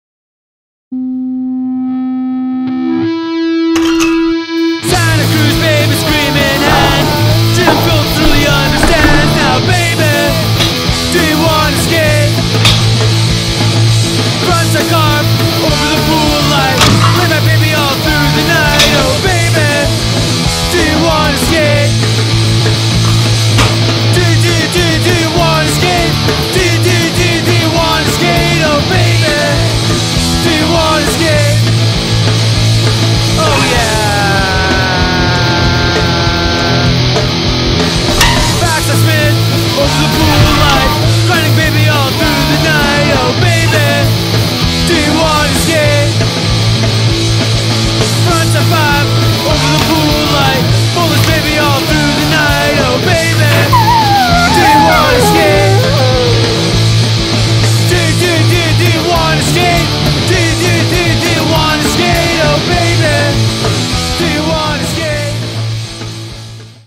Piano Skateboarding
Larry and Scott skate the Area 50-50 ramp and session the piano bank.